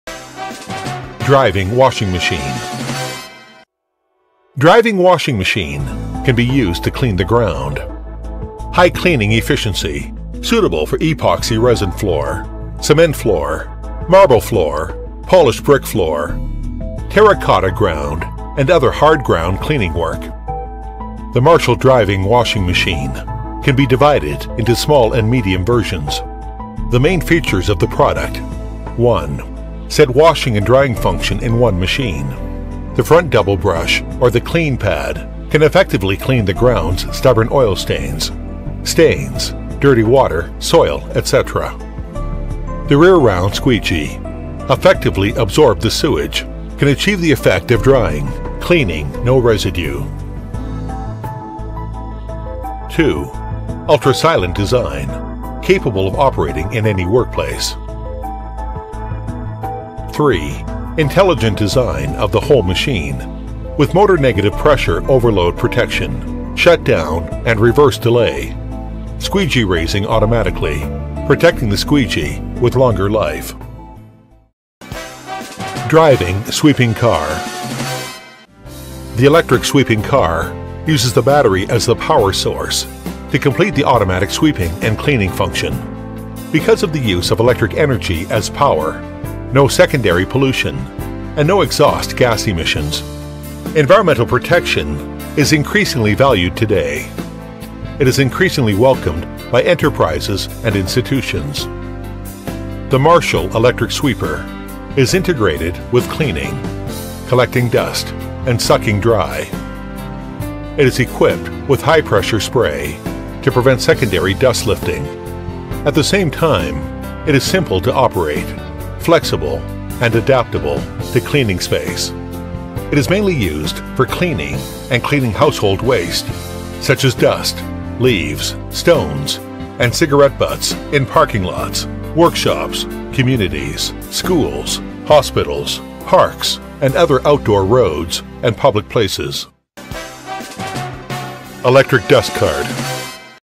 Marshell Electric Driving Washing Vehicle
Large tank capacity, Ultra-quiet design, Brush attached to a pressurized system tray, suitable for a variety of work environments